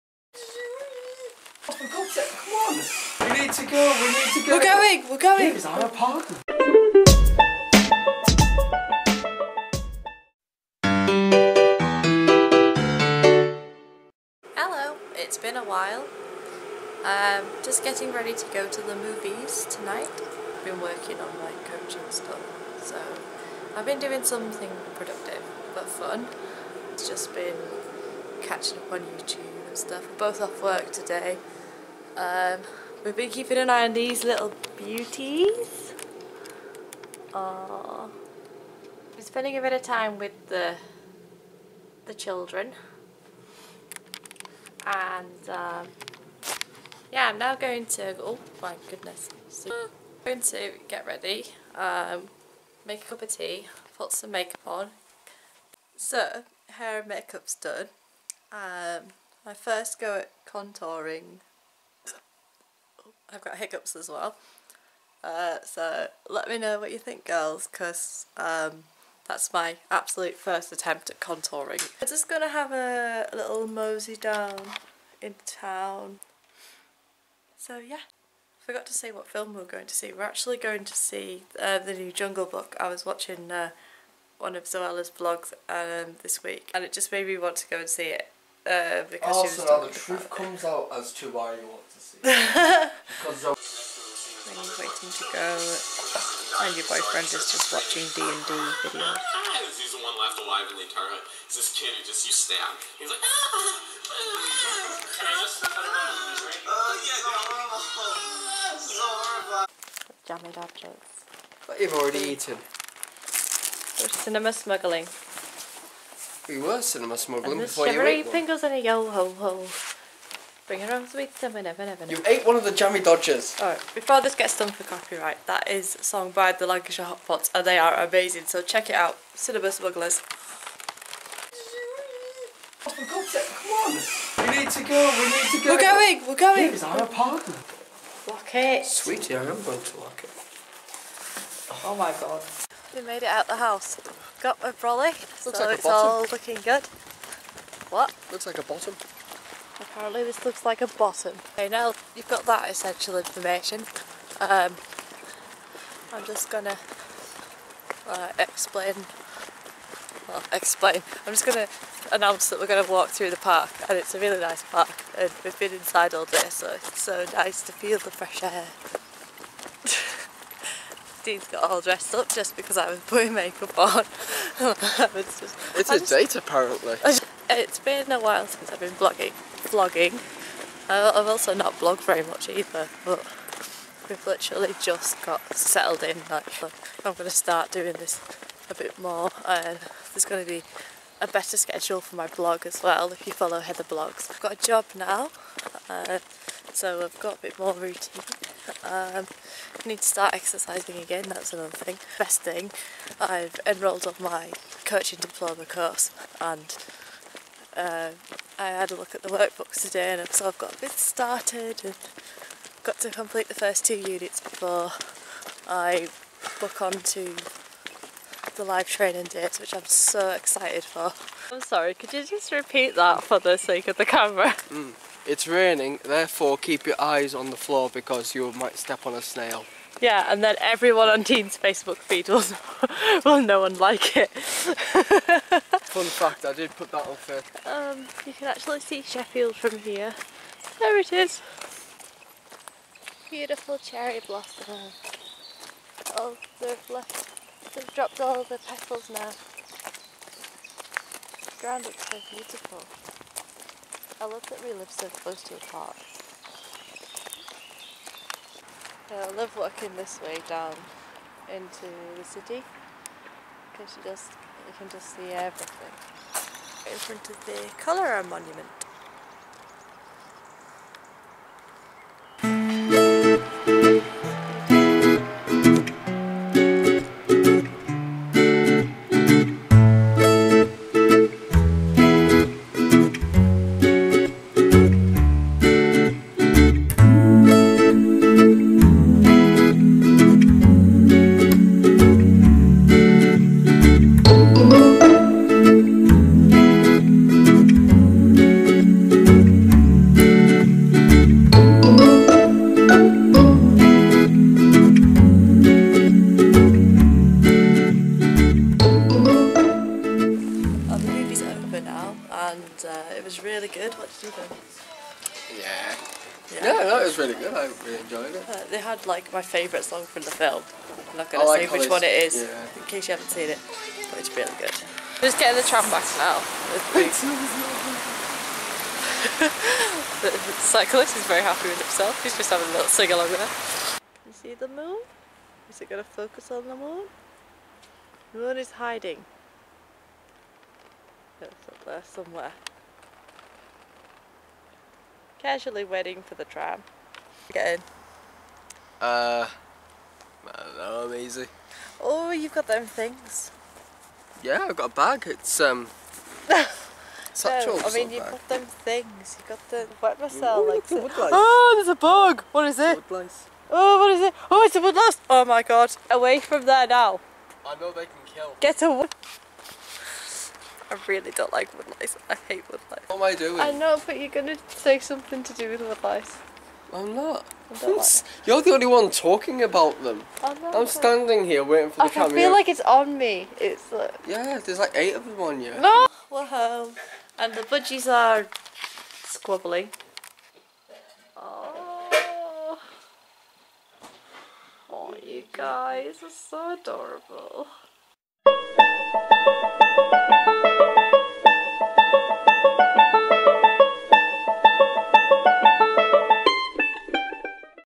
MID WEEK MOVIE DATE
Random mid week movie date to see the new Jungle Book!